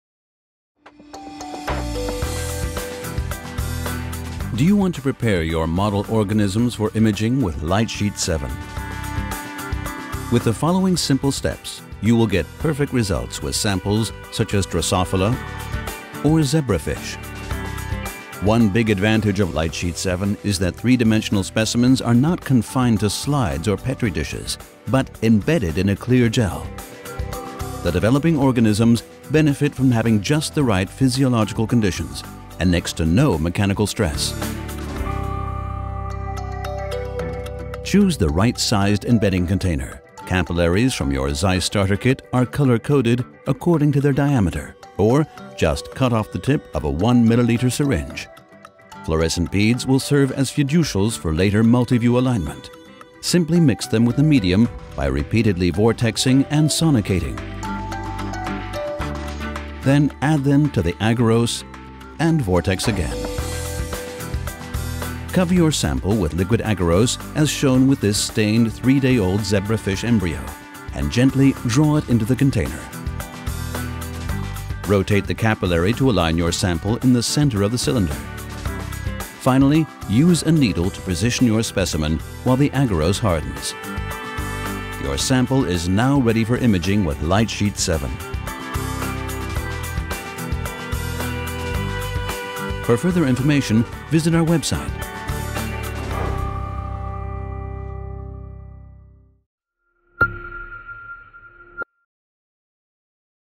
ZEISS Lightsheet 7 - Sample Preparation Guide
See in this video a sample preparation guide for light sheet fluorescence imaging with ZEISS Lightsheet 7.